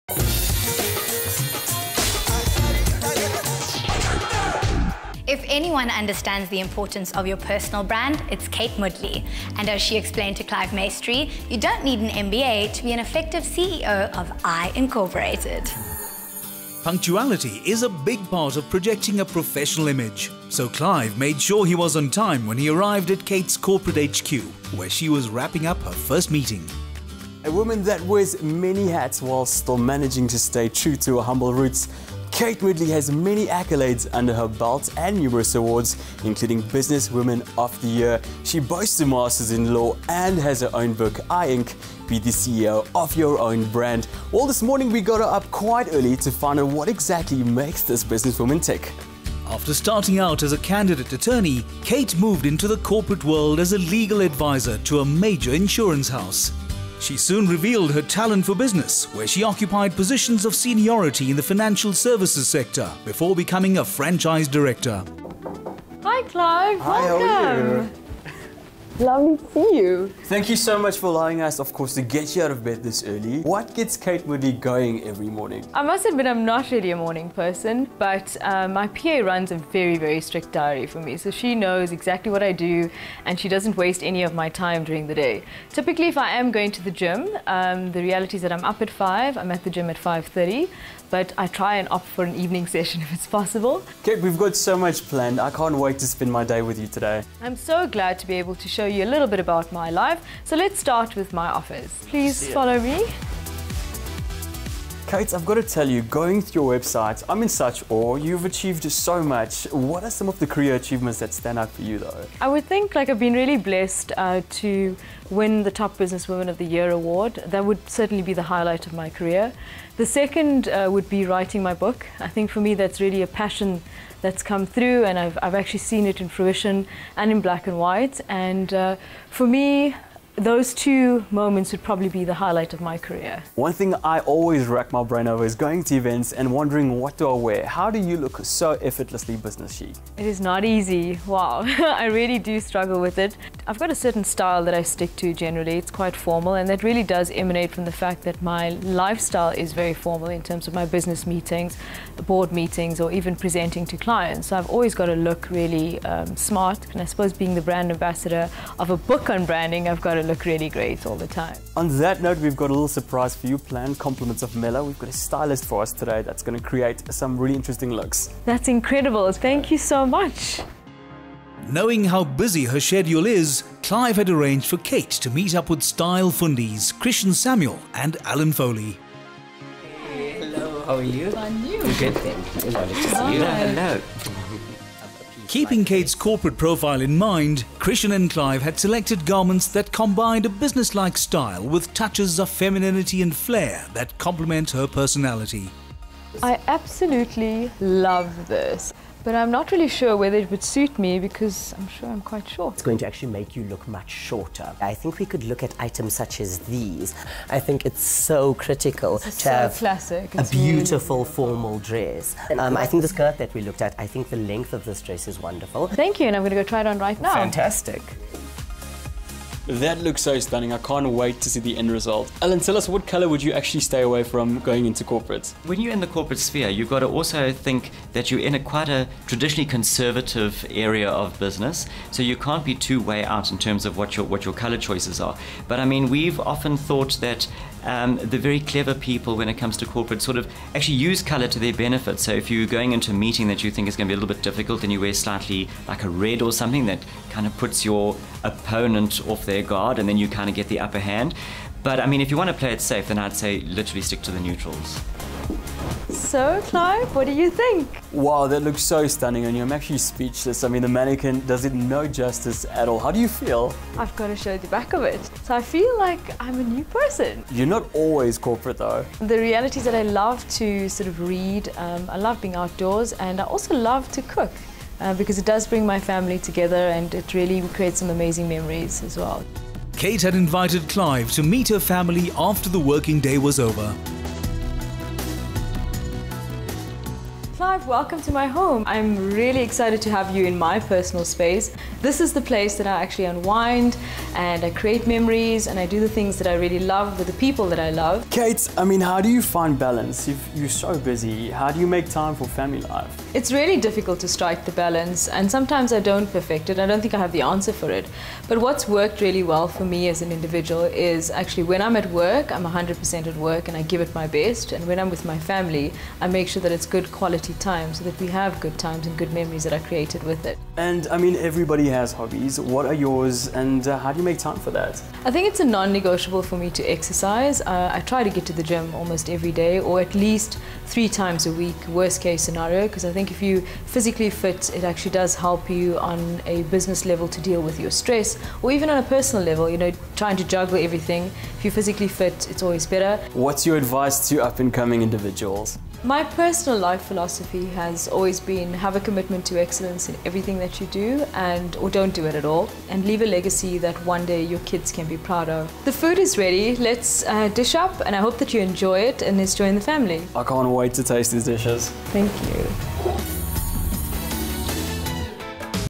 Kate Moodley : Businesswoman & Author of "I Inc."
If anyone understands the importance of your personal brand it’s Kate Moodley – and as she explained to Clive Maistry, you don’t need an MBA to be an effective CEO of I Incorporated.